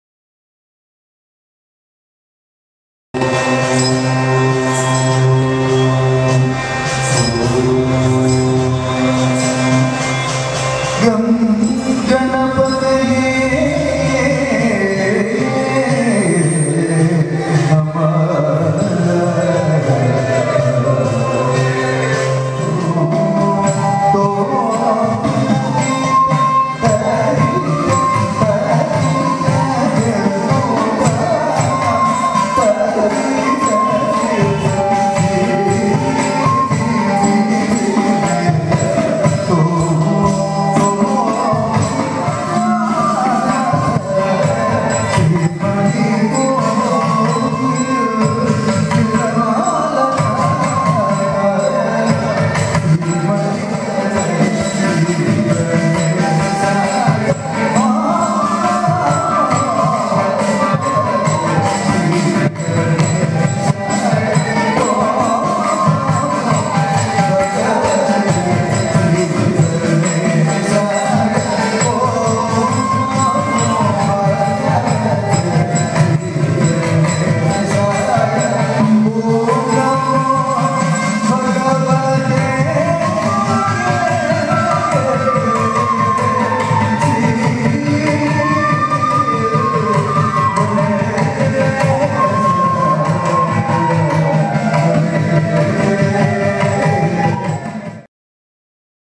Mohiniyattam - Indian Classical Dance from Kerala
Mohiniyattam by Dhanya Ajith and team at Centenary celebrations of  Kalamandalam Krishnan Nair and Kalamandalam Kalyanikutty Amma,the maestros of Indian Classical Dance - Kathakali & Mohiniyattam. On 11 April 2014 at Thrippunithura, Ernakulam, Kerala.